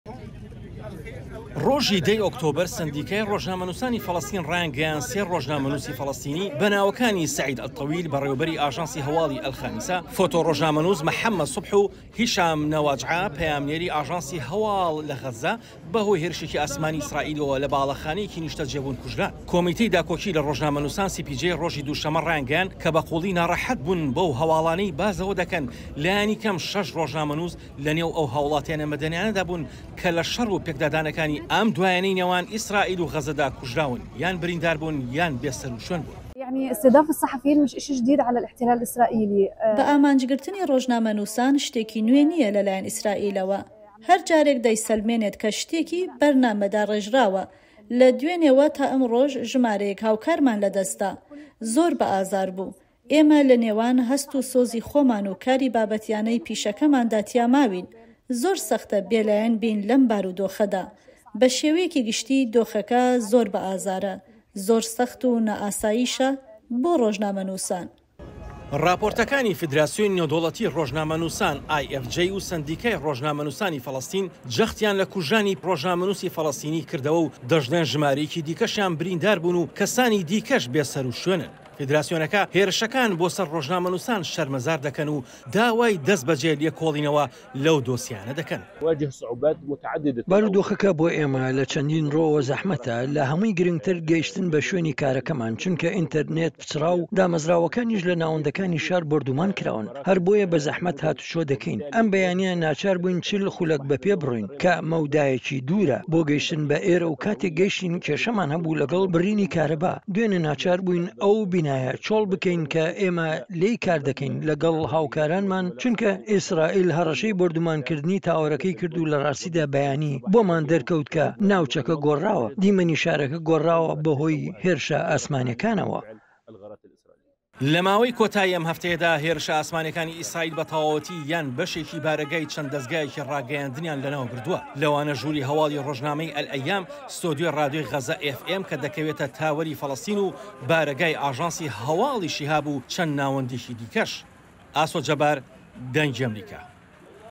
دەنگی ئەمەریکا VOA | کوژران و بێسەروشوێن بوونی چەند ڕۆژنامەوانێک لە شەڕی نێوان ئیسرائیل و حەماسدا
لیژنەی پاراستنی ڕۆژنامەنووسان ڕایگەیاند بە قووڵی نیگەرانن سەبارەت بەو هەواڵانەی کە باس لەوە دەکەن لانیکەم شەش ڕۆژنامەنووس لەنێو ئەو هاووڵاتییە مەدەنیانەدان کە لە شەڕی بەردەوامی نێوان ئیسرائیل و حەماسدا کوژراون یان بریندار بوون یانیش بێسەروشوێن بوون.